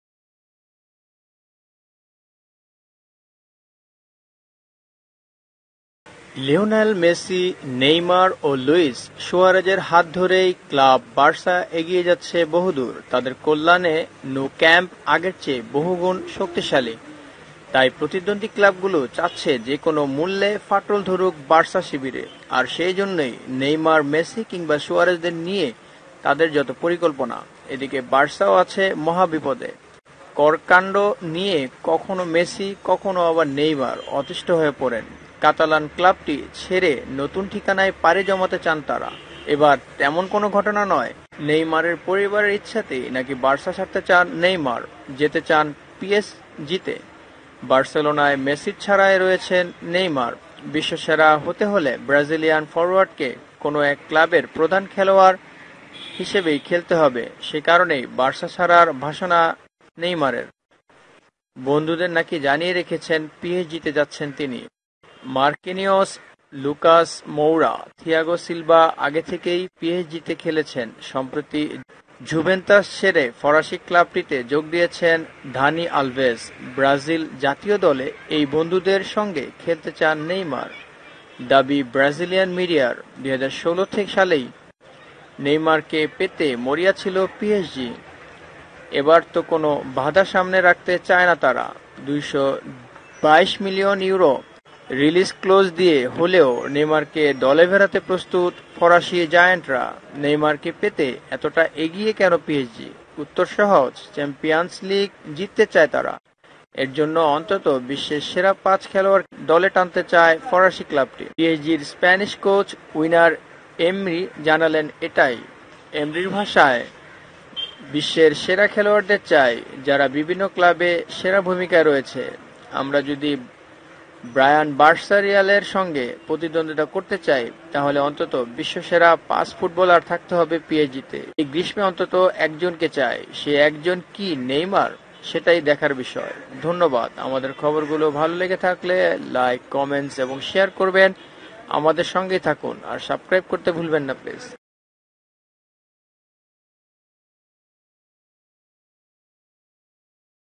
বাবার লোভের কারনেই বার্সা ছাড়ছে নেইমার যোগ দিচ্ছে পিএসজিতে /Neymar
neymer barcelona chere Psg te বাবার লোভের কারনেই বার্সা ছাড়ছে নেইমার যোগ দিচ্ছে পিএসজিতে,Neymar
ক্রাইম সংবাদ
লিওনেল মেসি, নেইমার ও লুইস সুয়ারেজের হাত ধরে ক্লাব বার্সা এগিয়ে যাচ্ছে বহুদূর। তাদের কল্যাণে ন্যু ক্যাম্প আগের চেয়ে বহুগুণ শক্তিশালী। তাই প্রতিদ্বন্দ্বী ক্লাবগুলো চাচ্ছে যে কোন মূল্যে ফাটল ধরুক বার্সা শিবিরে। আর সে জন্যই নেইমার-মেসি কিংবা সুয়ারেজদের নিয়ে তাদের যত পরিকল্পনা।

এদিকে বার্সাও আছে মহা বিপদে! কর-কাণ্ড নিয়ে কখনো মেসি, কখনও আবার নেইমার অতিষ্ট হয়ে পড়েন। কাতালান ক্লাবটি ছেড়ে নতুন ঠিকানায় পাড়ি জমাতে চান তারা। এবার তেমন কোনো ঘটনা নয়। নেইমারের পরিবারের ইচ্ছাতেই নাকি বার্সা ছাড়তে চান নেইমার। যেতে চান পিএসজিতে।

বার্সেলোনায় মেসির ছায়ায় রয়েছেন নেইমার। বিশ্বসেরা হতে হলে ব্রাজিলিয়ান ফরোয়ার্ডকে কোনো এক ক্লাবের প্রধান খেলোয়াড় (প্রাণভোমরা) হিসেবেই খেলতে হবে। সে কারণেই বার্সা ছাড়ার বাসনা নেইমারের! বন্ধুদের নাকি জানিয়ে রেখেছেন, পিএসজিতে যাচ্ছেন তিনি।

মারকিনিয়োস, লুকাস মৌরা, থিয়াগো সিলভা আগে থেকেই পিএসজিতে খেলছেন। সম্প্রতি জুভেন্তাস ছেড়ে ফরাসি ক্লাবটিতে যোগ দিয়েছেন দানি আলভেজ। ব্রাজিল জাতীয় দলের এই বন্ধুদের সঙ্গে খেলতে চান নেইমার। দাবি ব্রাজিলিয়ান মিডিয়ার।

২০১৬ সালেই নেইমারকে পেতে মরিয়া ছিল পিএসজি। এবার তো কোনো বাধা সামনে রাখতে চায় না তারা। ২২২ মিলিয়ন ইউরোর রিলিজ ক্লজ দিয়ে হলেও নেইমারকে দলে ভেড়াতে প্রস্তুত ফরাসি জায়ান্টরা।

নেইমারকে পেতে এতটা এগিয়েছে কেন পিএসজি? উত্তর সহজ- চ্যাম্পিয়ন্স লিগ জিততে চায় তারা। এর